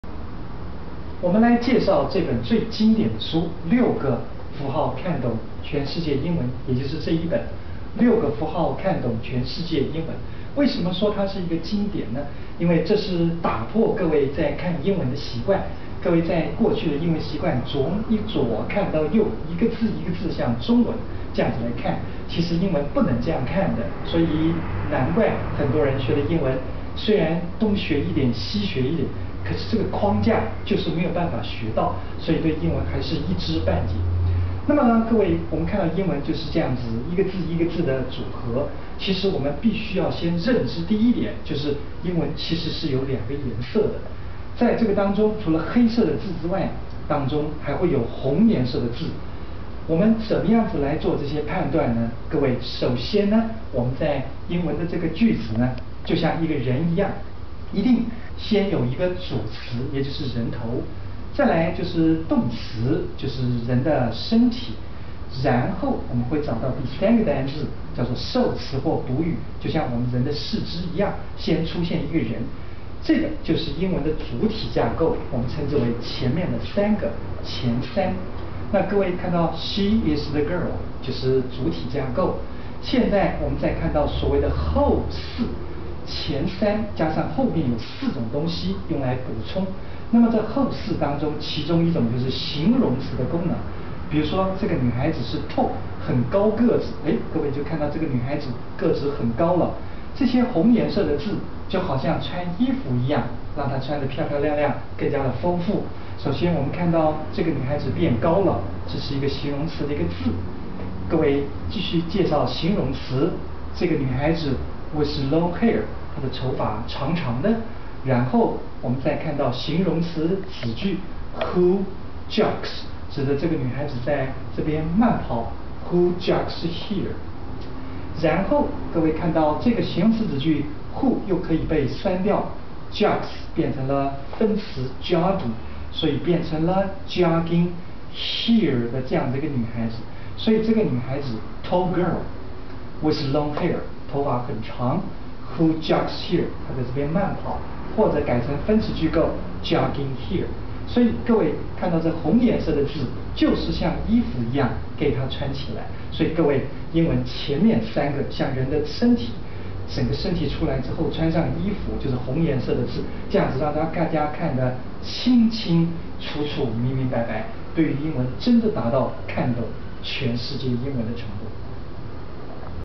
6個符號英文法-推薦書籍-6個符號看懂全世界英文
本書提供學英文最好的方法：即用六個符號來貫通全部英文文法結構。這是英語教學碩士─仲華老師根據多年教學經驗，結合美式英語教學法則所獨創的英文學習方法。在學習英文時，先利用六個符號將那句子分成一小節、一小節的，使句子變得簡單易懂，進而了解其文章結構，再透過CD帶領讀者不斷朗誦，加深印象並記憶其單字用法。如此便可輕鬆了解英文文法結構，進而加強讀者聽,說,讀,寫的能力。